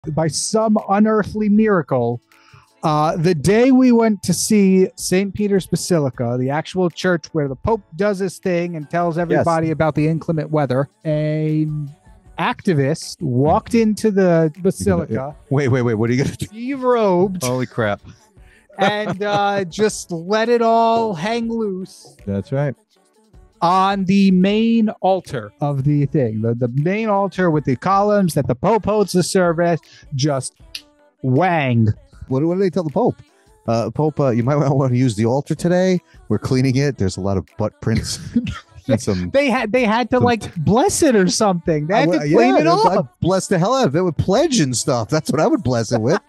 The Vatican P3N1S Man (A True Story)
On June 1st, a man jumped onto the main altar of St. Peter's Basilica and derobed. Your duck feeders just happened to be at the Vatican (the one in Rome) on the same day. What are the odds?